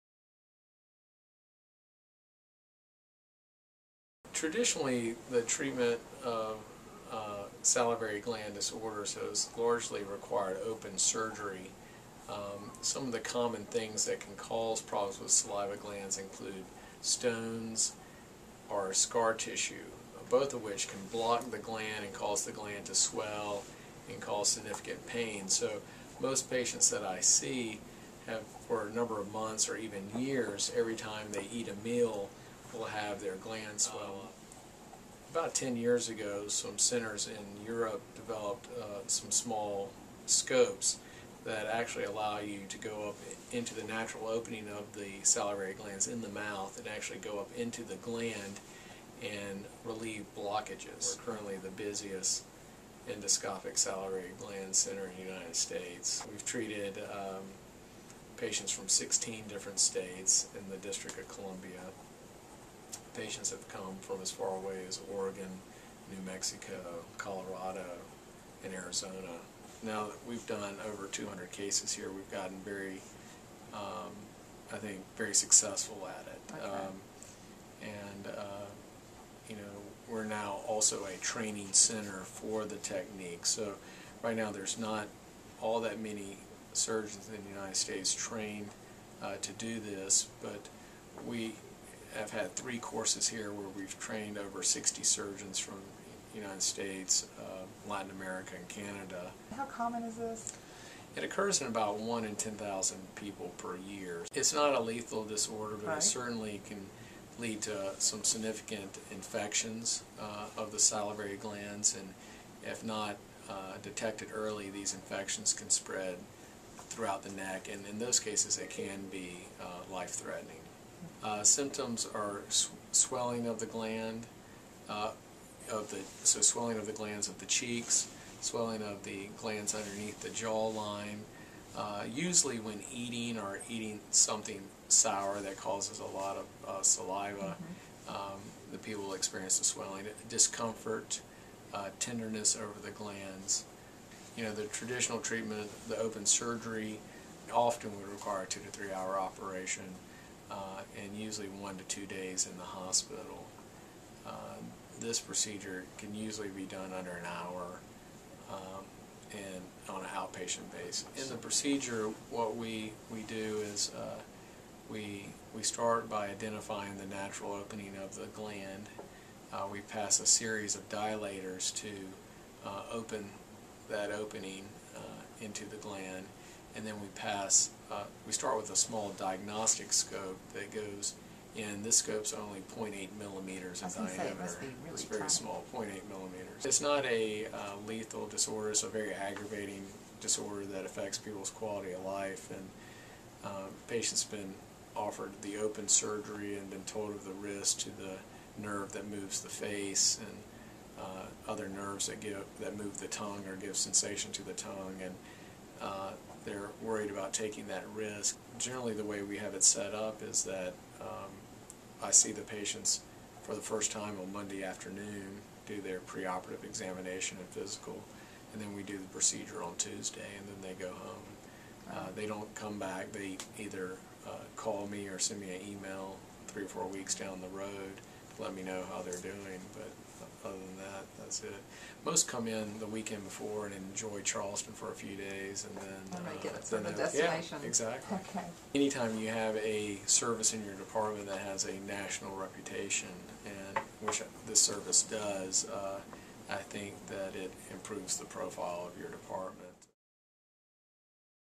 MUSC's Dr. M. Boyd Gillespie and Salivary Gland Surgery
Dr. Gillespie has made MUSC the leading institution in the country in minimally evasive treatment of salivary gland disorders.